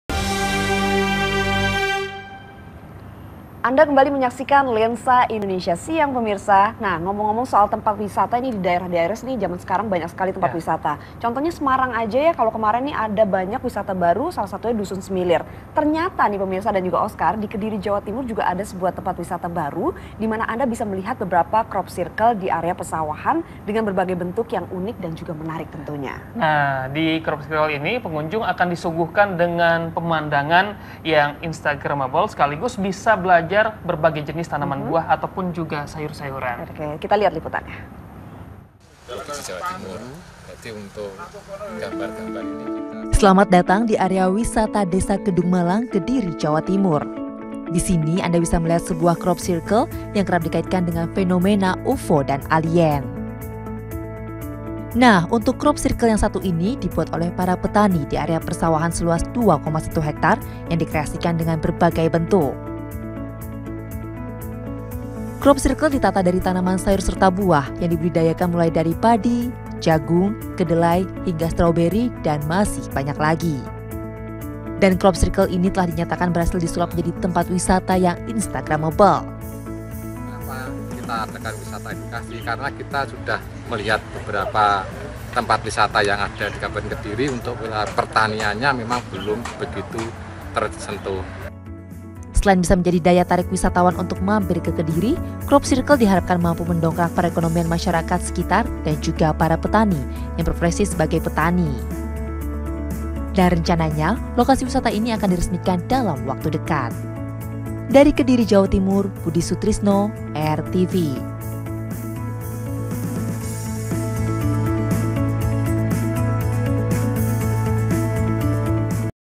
Wisata Crop Cirle di Sawah Petani Kediri Jawa Timur
di Kediri ada sebuah tempat wisata baru dimana kamu bisa melihat beberapa crop circle di area persawahan dengan bentuk yang unik